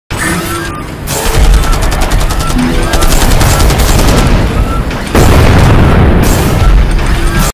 Kimweri Warzone (blender, Element 3d, After Effects)
Pod bots attack Dar es Salaam, Tanzania!